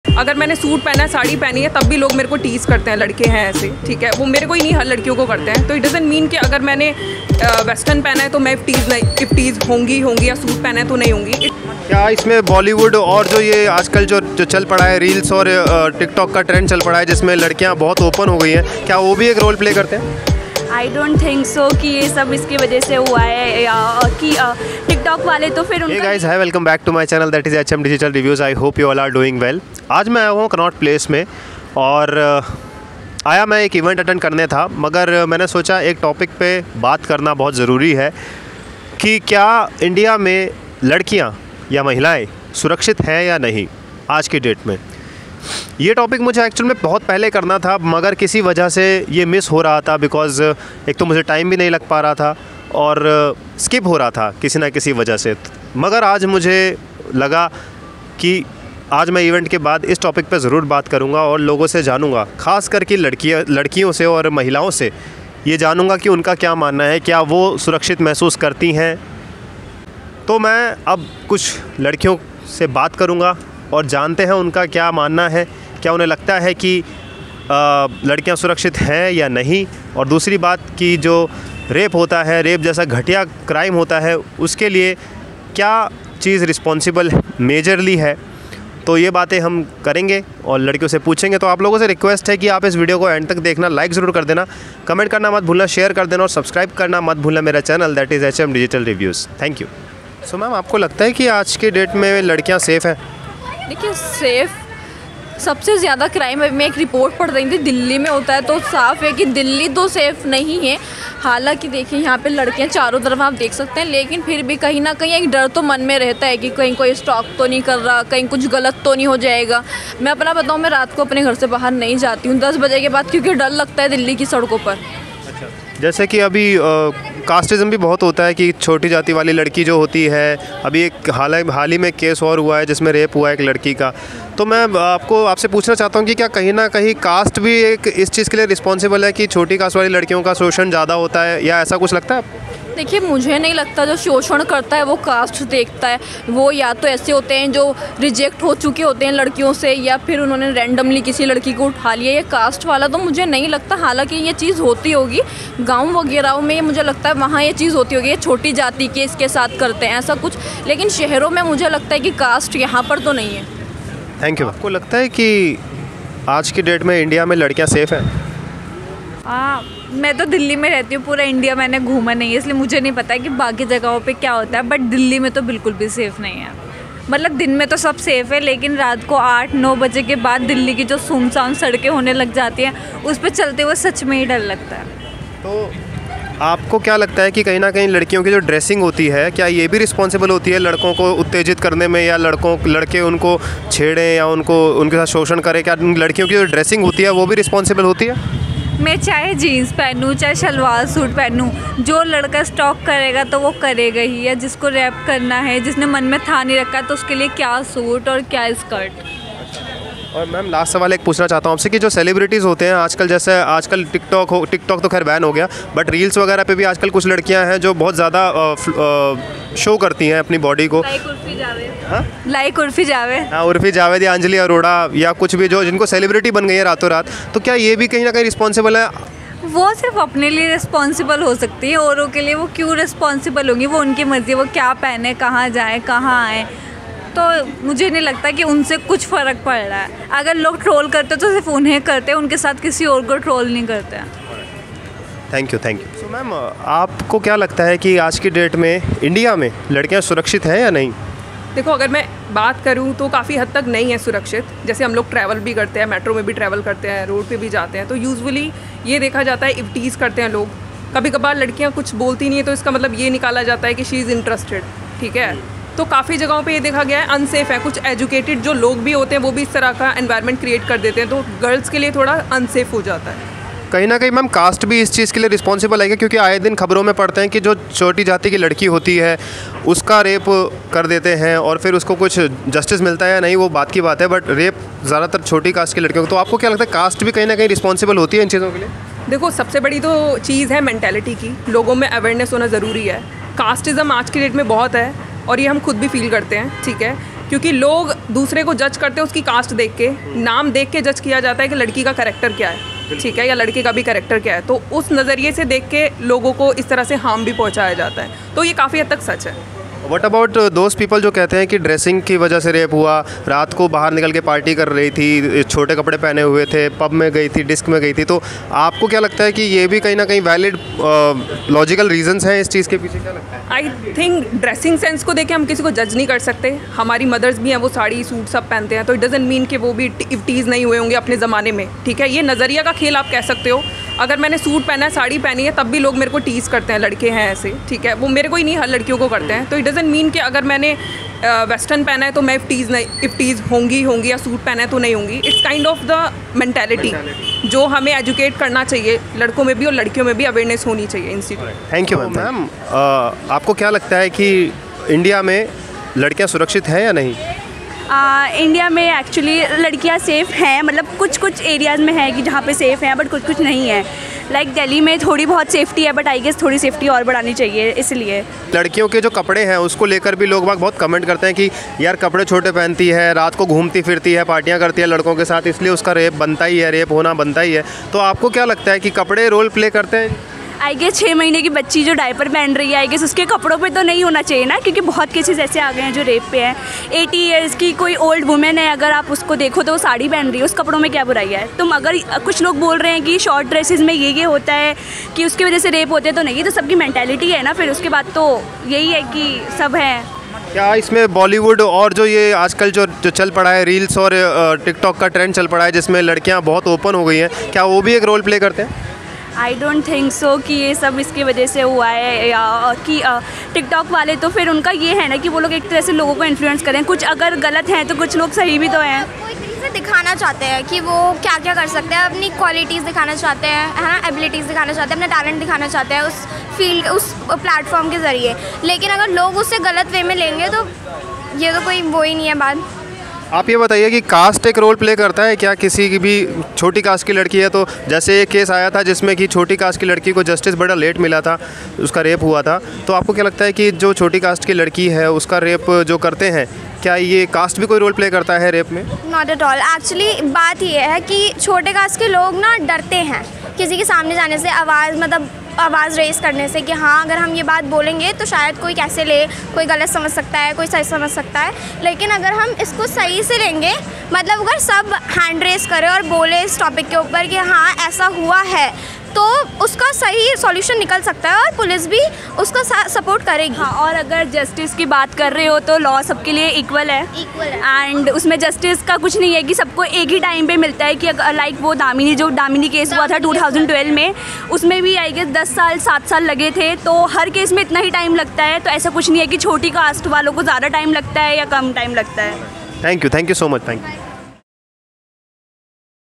Delhi girls speak about rape, molestation and Eve Teasing || Public Review || Women Safety
Today, I asked girls of Delhi about Women Safety in India and asked them about do they feel safe in India ??? Watch and find out what they said.